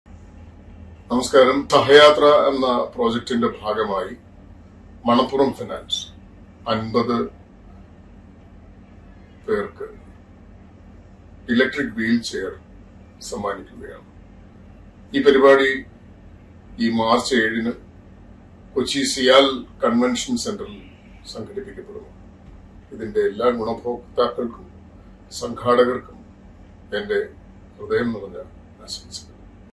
സഹയാത്രക്ക് ....സ്നേഹസ്പർശമായ്....മണപ്പുറം ഫൗണ്ടേഷൻ
*സഹയാത്രക്ക് .
സ്നേഹസ്പർശമായ്.
മണപ്പുറം ഫൗണ്ടേഷൻ*
- Distribution of 50 electric wheelchairs for muscular Dystrophy /Atrophy Patients from economically backward families.
Venue: CIAL CONVENTION CENTRE
Date: 7th March 2023
Time : 3 pm to 7 pm